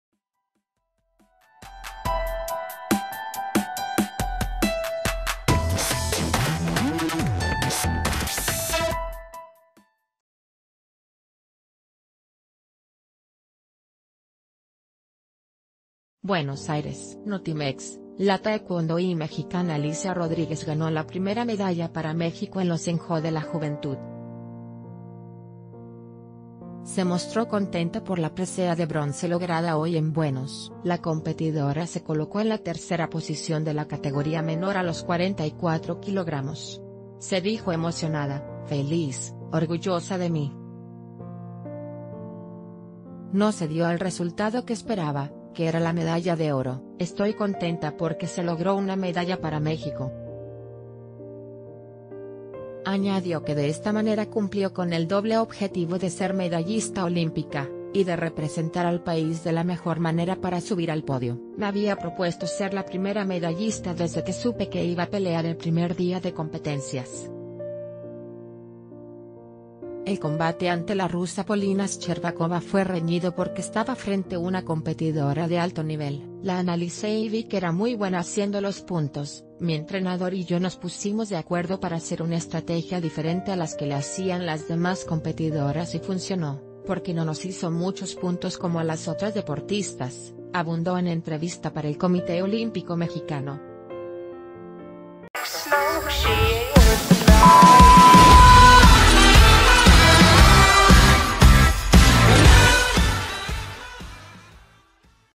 Taekwondoí da primera medalla para México en los en JO de la Juventud - El Diario de Yucatán
BUENOS AIRES, (Notimex). — La taekwondoí mexicana Alicia Rodríguez ganó la primera medalla para México en los en JO de la Juventud. Se mostró contenta por la presea de bronce lograda hoy en Buenos. La competidora se colocó en la tercera posición de la categoría menor a los 44 kilogramos. Se dijo “em...